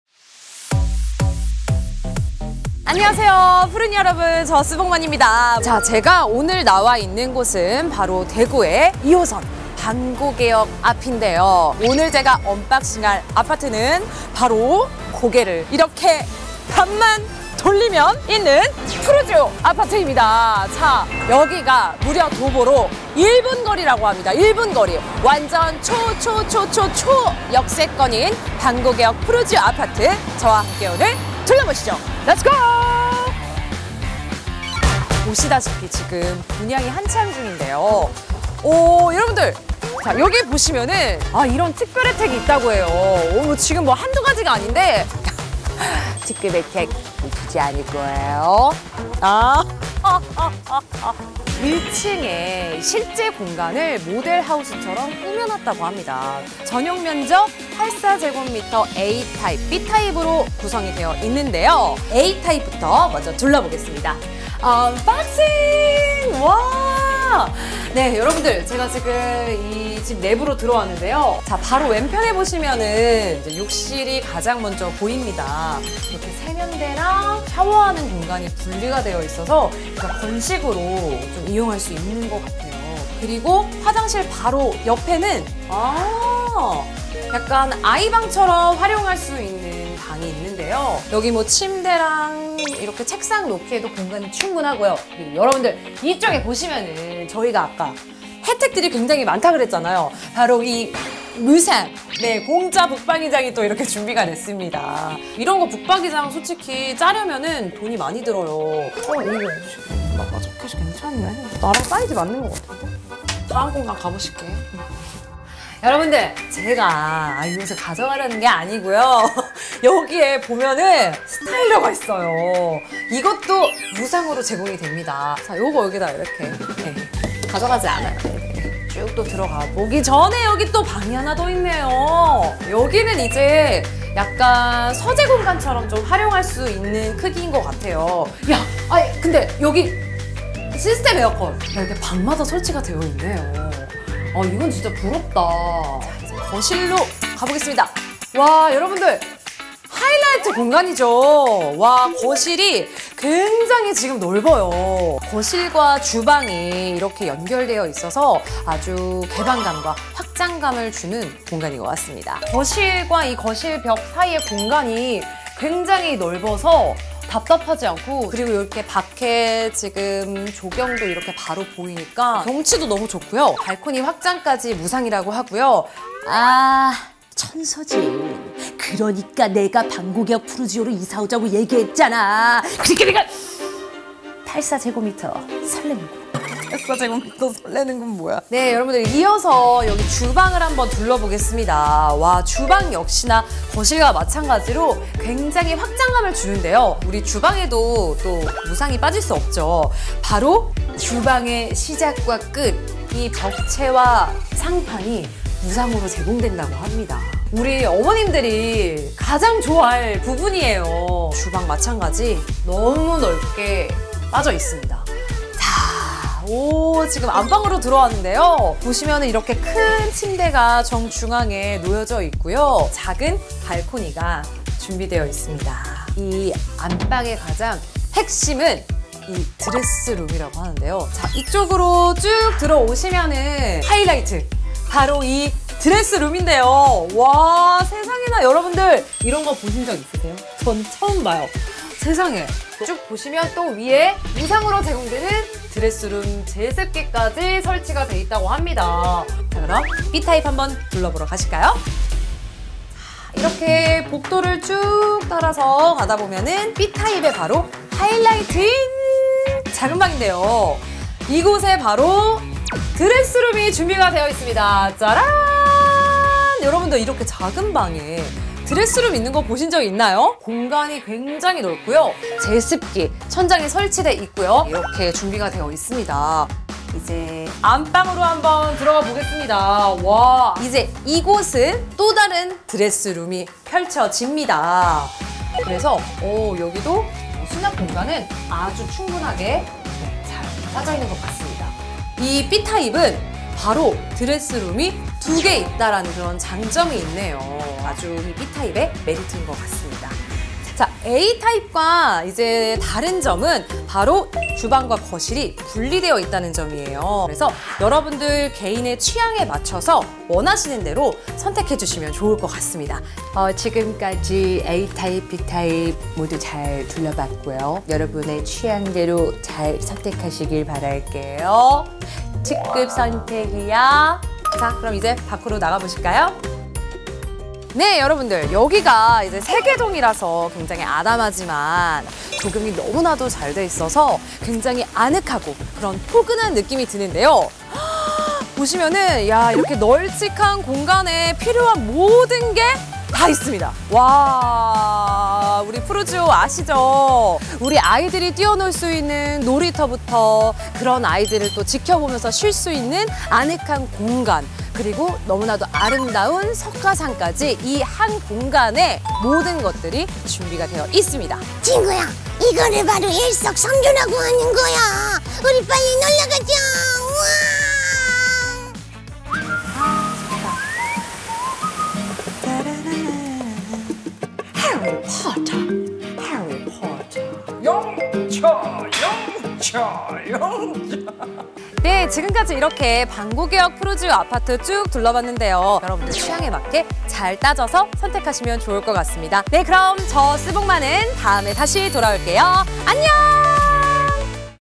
노른자 식땅 EP.32 반고개역 푸르지오 특별분양 대공개
EP. 32 🎀 반고개역 푸르지오 특별 분양 🎀

성대모사의 아이콘 쓰복만과 함께 하는
대구 반고개역 푸르지오 특별분양 소개

- 초역세권, 슬세권 등 프리미엄 인프라
- ⭐특별 혜택⭐ 무상으로 제공되는 발코니 확장, 붙박이장, 의류관리기 등
- 피트니스 클럽, 그리너리 스튜디오 등 다양한 커뮤니티 시설

84m²A와 84m²B 샘플 하우스를 구경하면서
반고개역 푸르지오의 특급 무상 혜택까지 보러 오세요!!👏🏻